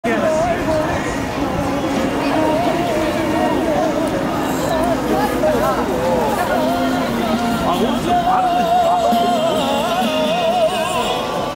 Korean Egg Bread
Let me introduce you to a popular Korean street snack gyeran-ppang (계란빵) or “egg bread” today. "Gyeran-ppang is a warm street snack sold throughout South Korea. The fluffy, oblong-shaped loaf of bread is sweet and savory with a whole egg inside the pancake dough."